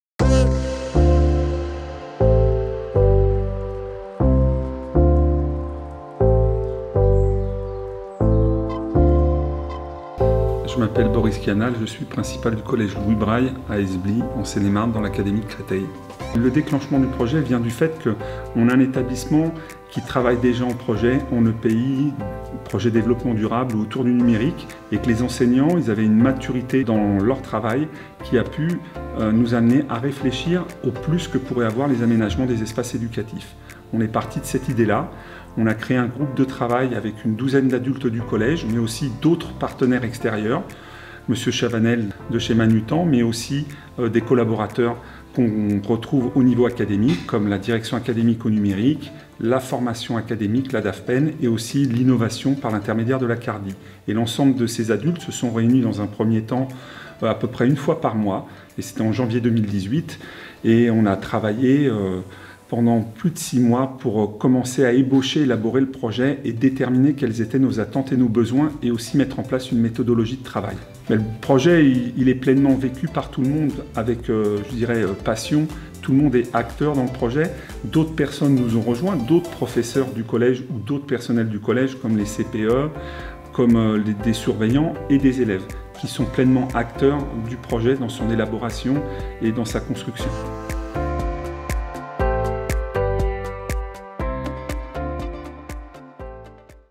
Témoignage Principale du collège d'Esbly - Boris Chianale - Aménagement de la Salle B208 (1/3)
Boris Chianale revient sur l'origine du lancement de l'aménagement des espaces éducatifs de la salle B208 avec les différents acteurs du projet